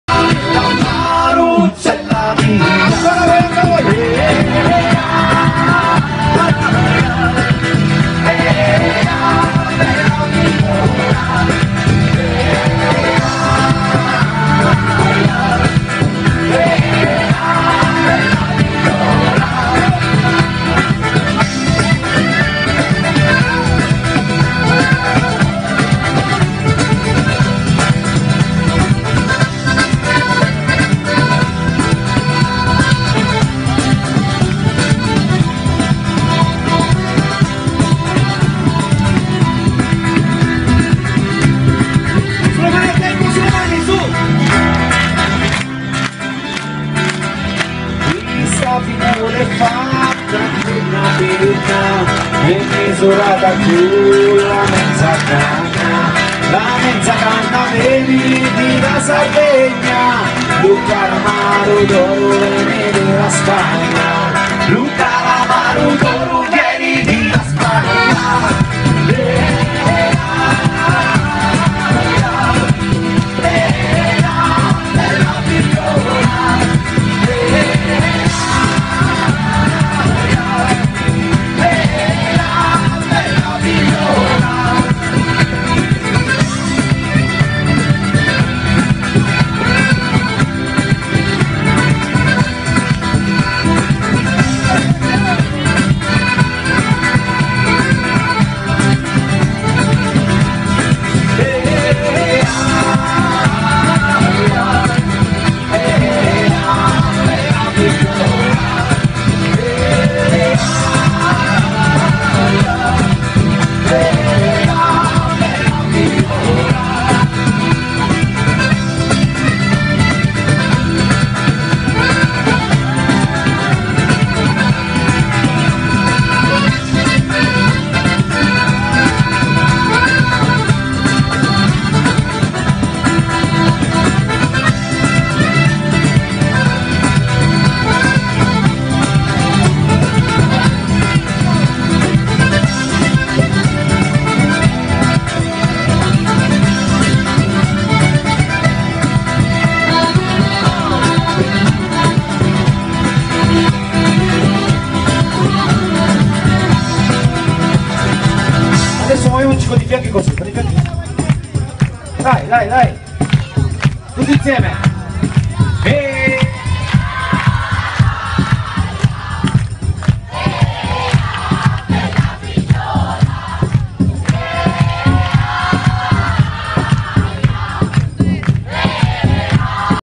Hosteria di Giò - Estate 2014 - Celico - Spagna
Concerto di fine estate 2014 a Celico.

HOSTERIA DI GIO'
Promotori della musica popolare calabrese nel mondo